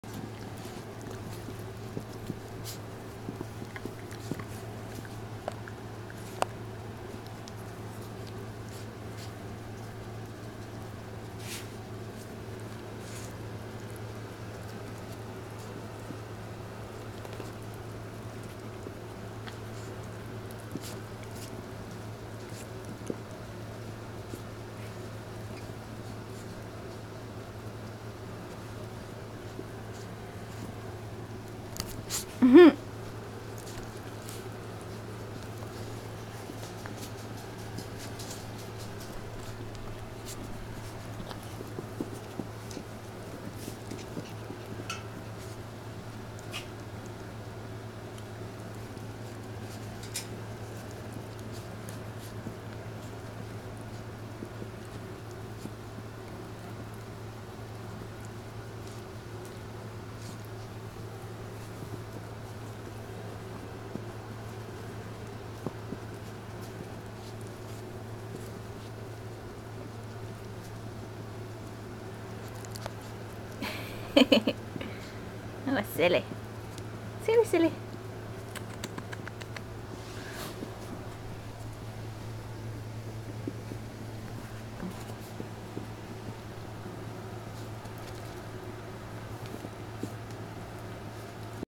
Teela trying to play - ADOPTED!
Teela love to play hard and needs to be reminded to relax, too.  She tries to play while her foster brother tries to bathe her.  She doesn't let him stop her.